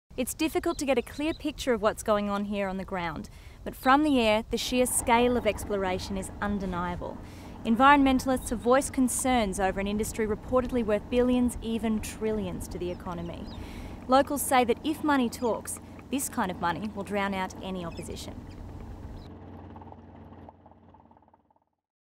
The scale of the operation
A rare look at coal seam and liquid natural gas exploration projects from the air, gives an idea of the sheer scale of operations in the Surat Basin. While looking at one well, consider that there are 54 thousand already marked in the area. Environmental groups such as Lock the Gate fear the impact of exploration around the Great Artesian Basin, which is referenced as Australia's water supply. The industry represents potential trillions of dollars - and it's thought no voice of dissent will stop the process -and that even if the gate is shut, the horse has already bolted.